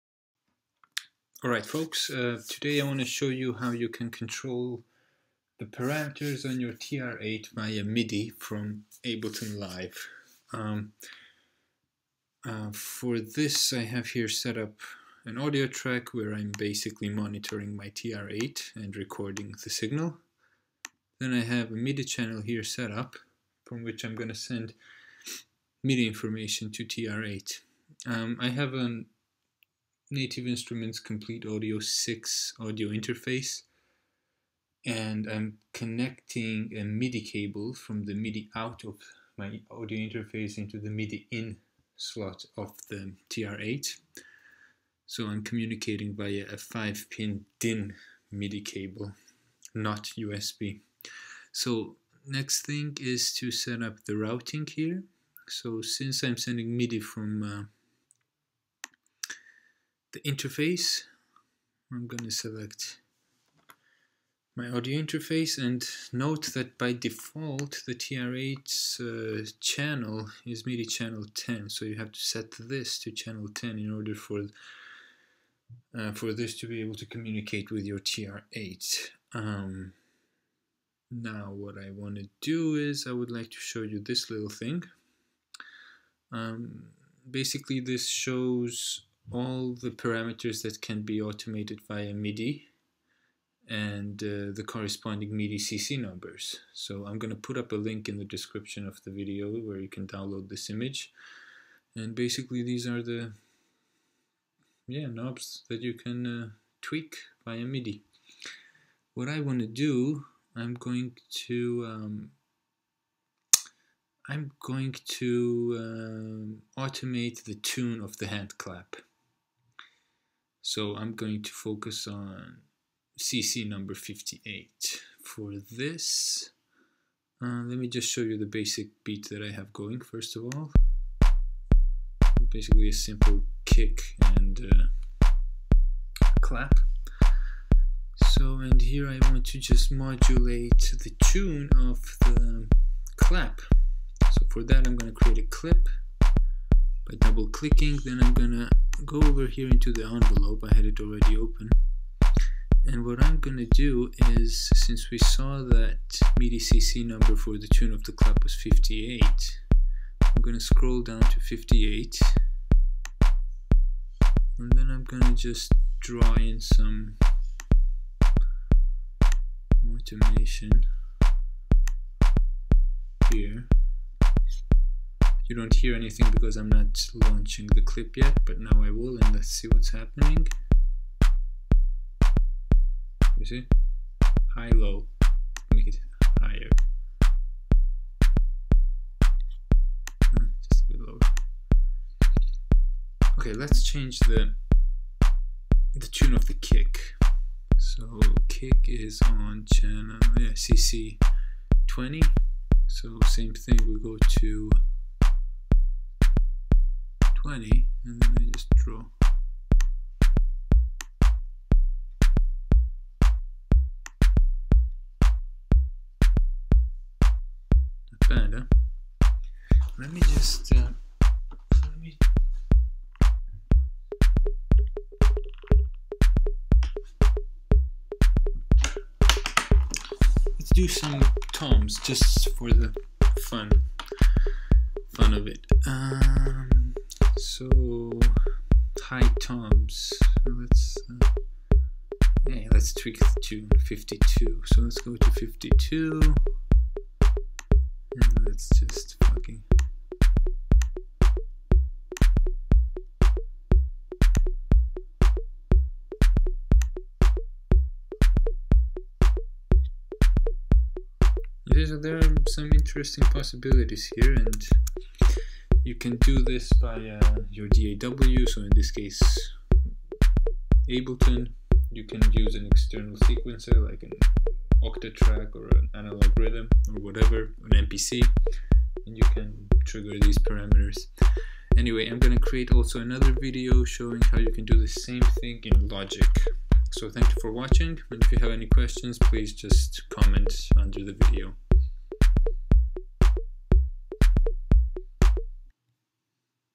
Control Roland TR8 Parameters Via MIDI in Ableton
A quick tutorial video showing how to control some parameters of the Roland TR8 drum machine via MIDI CC messages sent from Ableton Live.